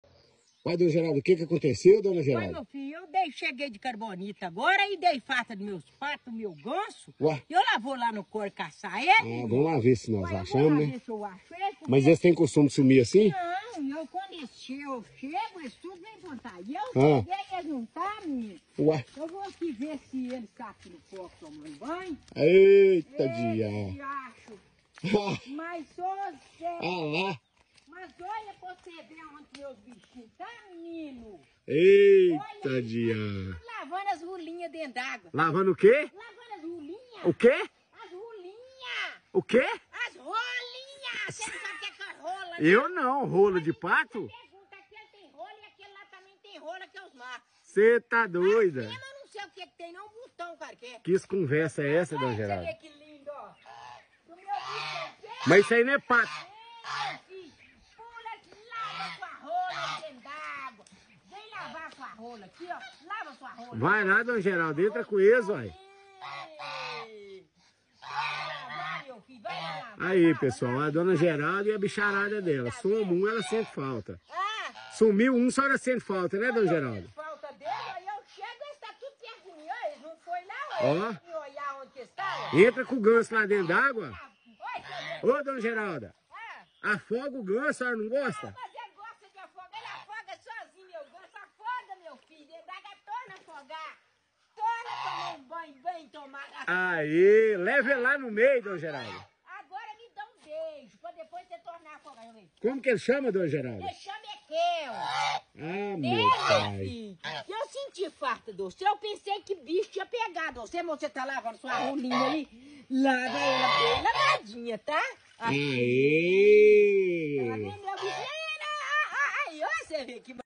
É impressionante como Dona Geralda se dá bem com sua bicharada!
Dona Geralda se dá muito bem com sua bicharada e parece que eles a entendem! É lindo de ser essa relação com os animais!bichos